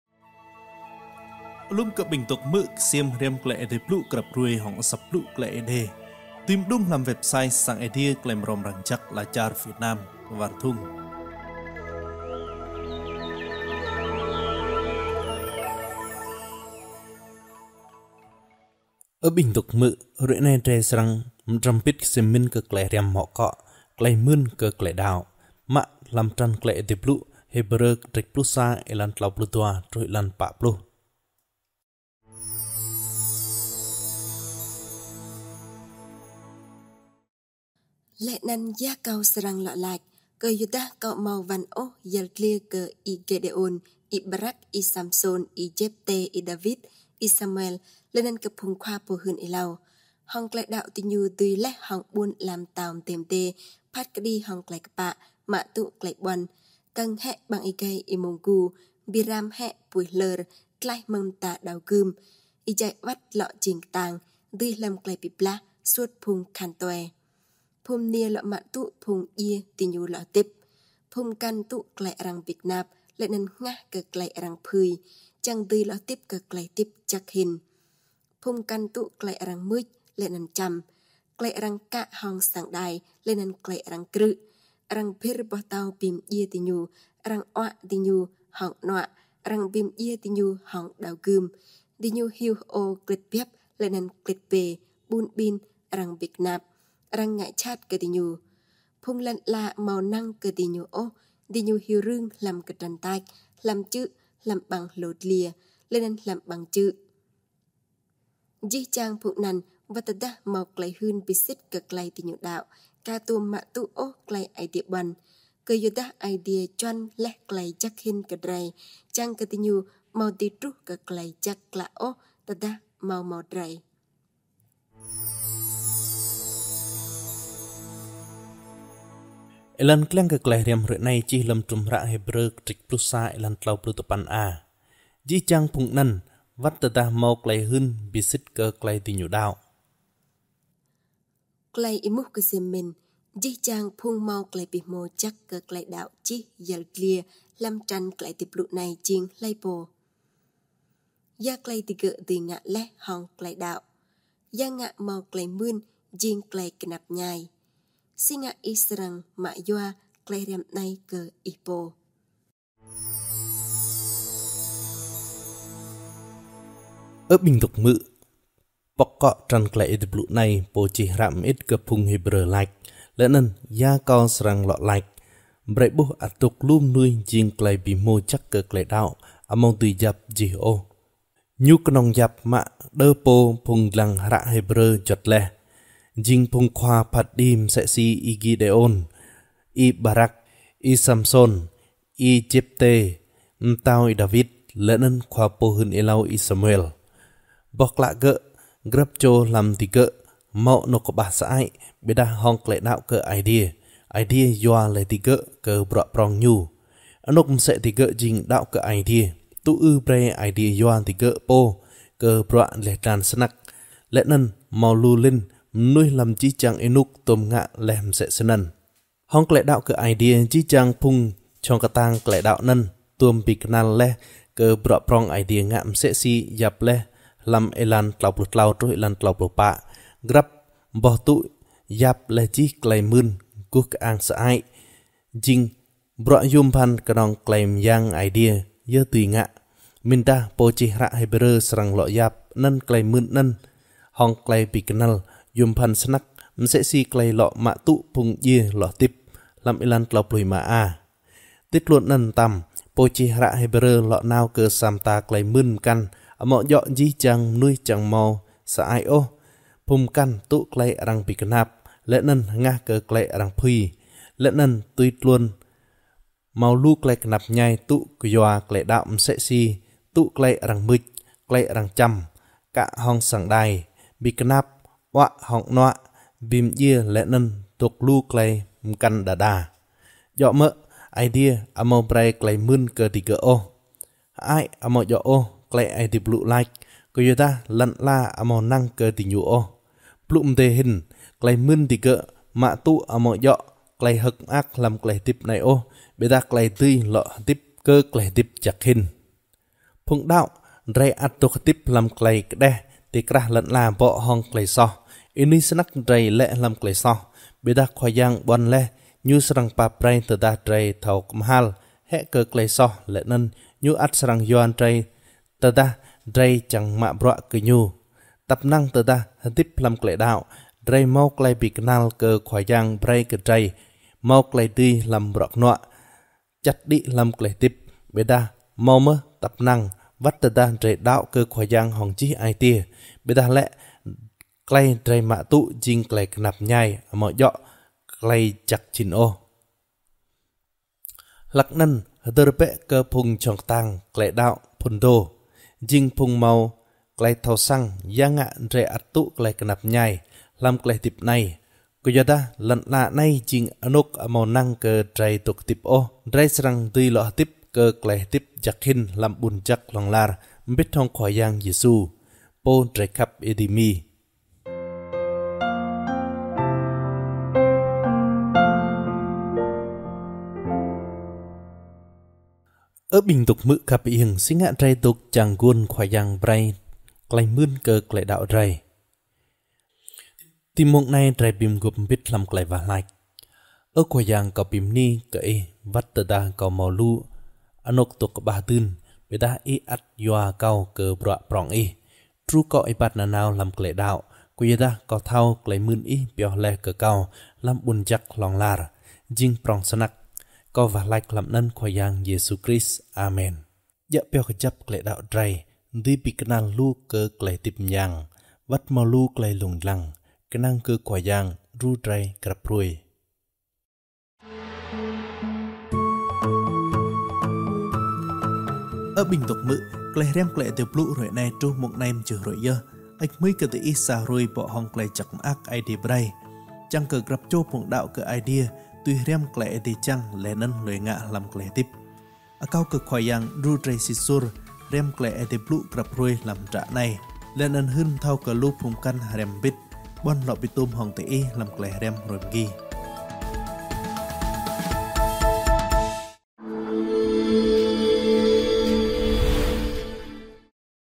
16/4/2024 BHKTHN TIẾNG ÊĐÊ
Hruê 16/4/2024
Klei Mưn Kơ Klei Đăo
Klei Aê Diê Blŭ: Hêbrơ 11:32-40
Êlan kliăng: “Jih jang phung anăn, wăt tơdah mâo klei hưn bi sĭt kơ klei diñu đăo…” (Hêbrơ 11:39a)